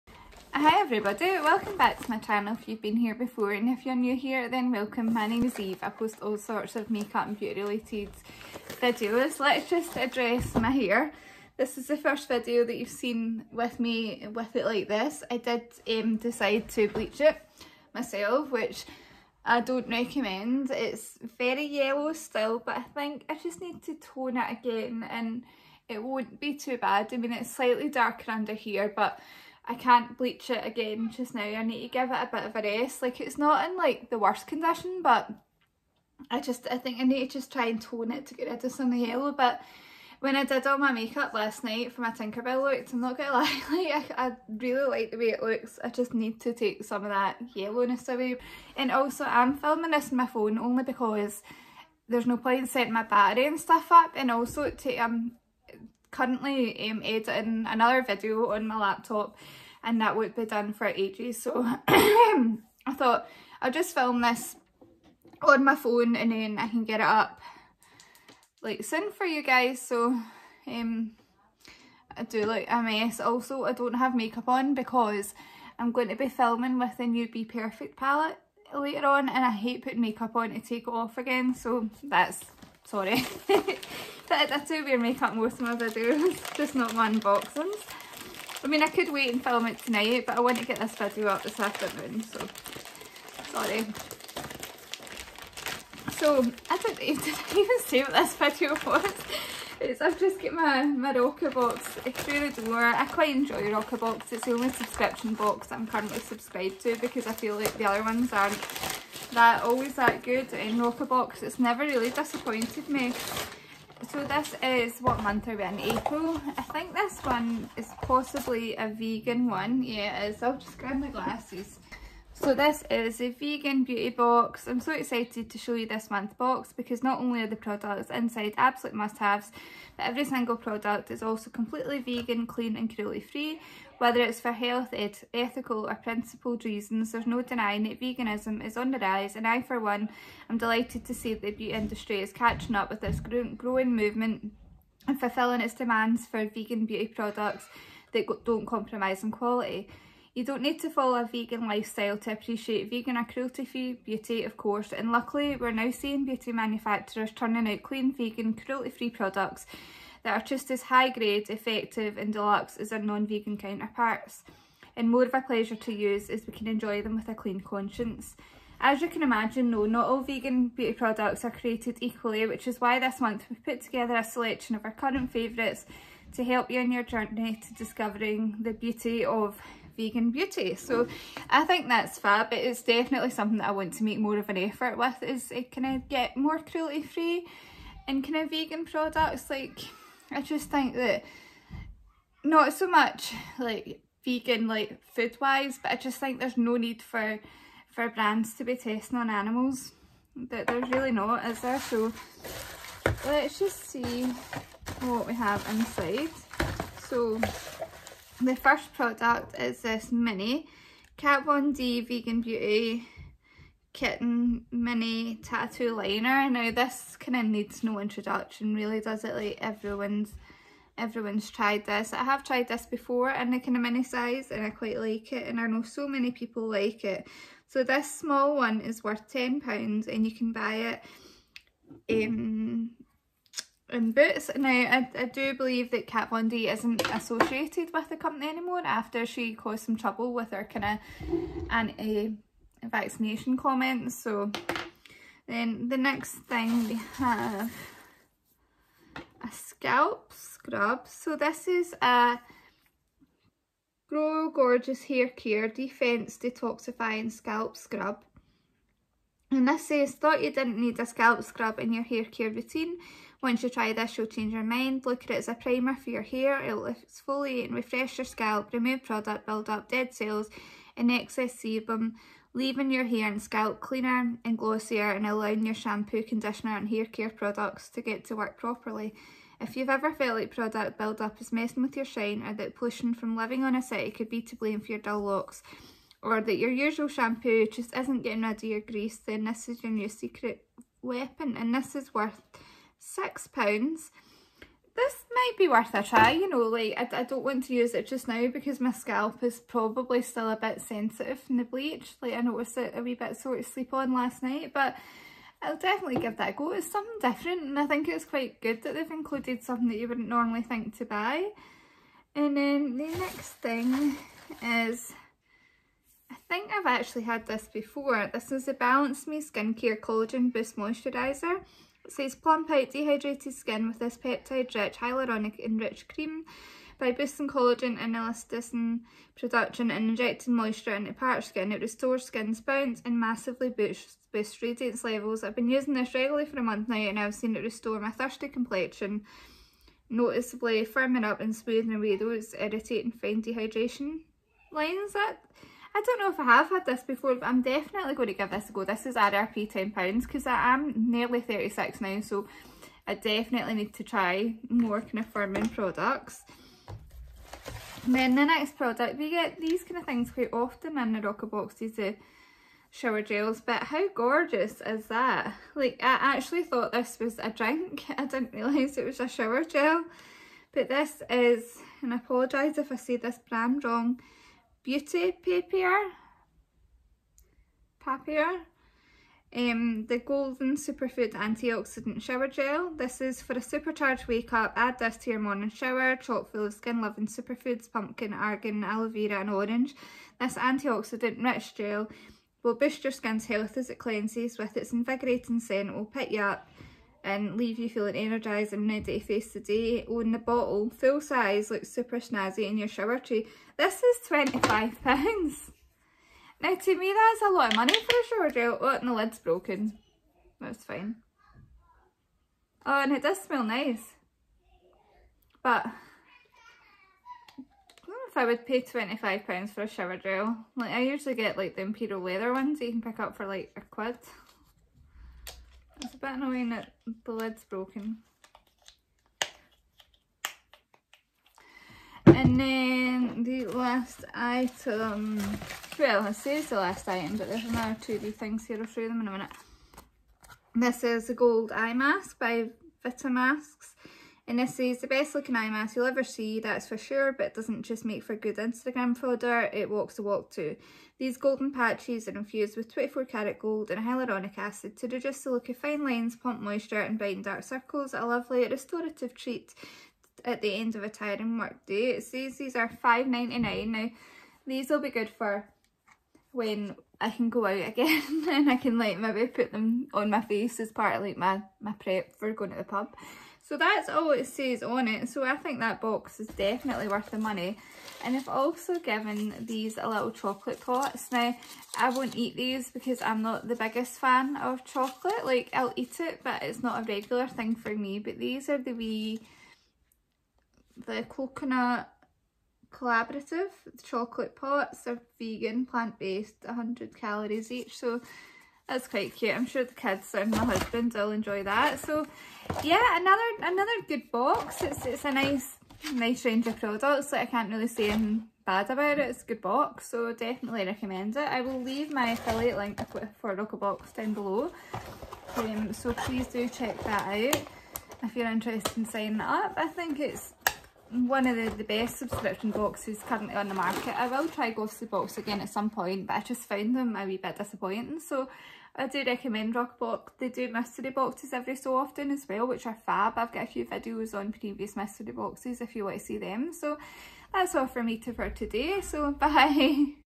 Roccabox April 2021 unboxing- Vegan edition
Hi everyone and welcome to today’s video!
This is an unboxing of the April 2021 Roccabox. This months box is all vegan and has a lovely selection of products.